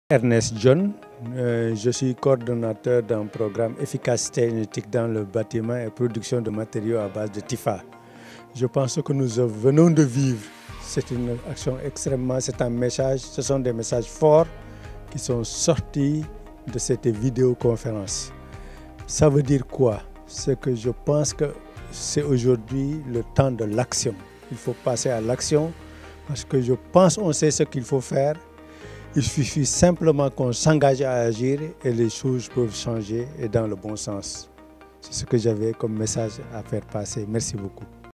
Sommet Action Climat. Ernest Ndione, Coordonnateur Projet PNEEB/TYPHA (Sept. 2019)
Sommet Action Climat. Ernest Ndione, Coordonnateur Projet Efficacité Energétique dans le Bâtiment (PNEEB/TYPHA). Ministère de l'Environnement et du Développement durable  (Sept. 2019)